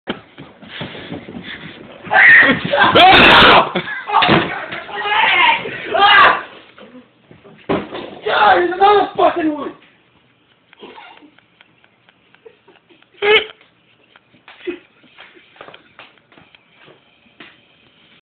Bug Attack 2
Zac chases Josh outside with a roach and throws it at him hitting him in the leg.